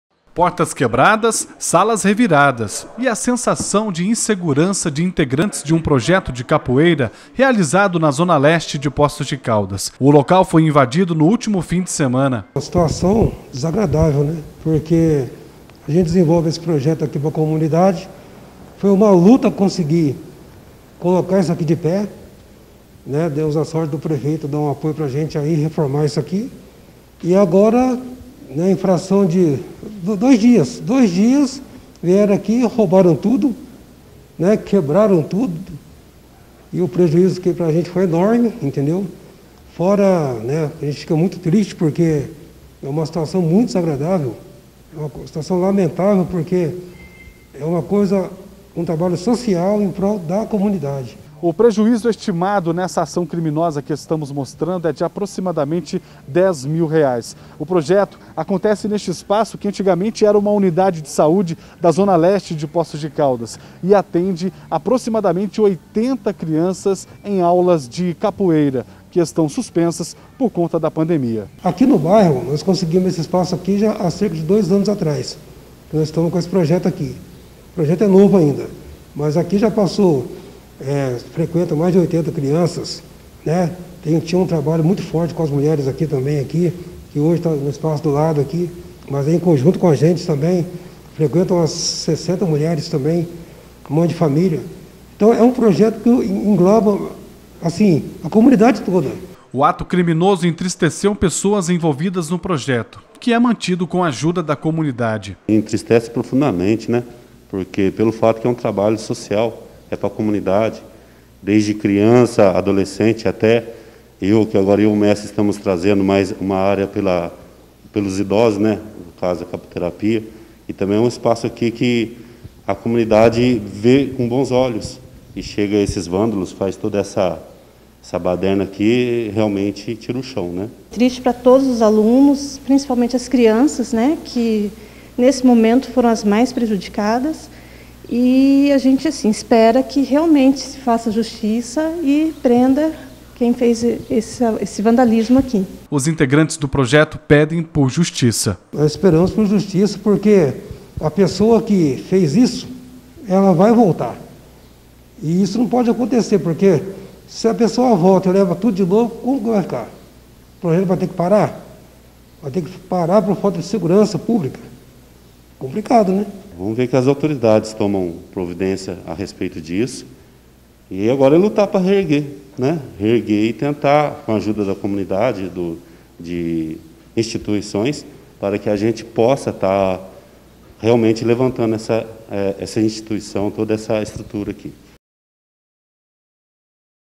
INVASÃO PROJETO DE CAPOEIRA
Local que realiza projeto de aulas de capoeira é invadido e tem prejuízo estima de R$ 10 mil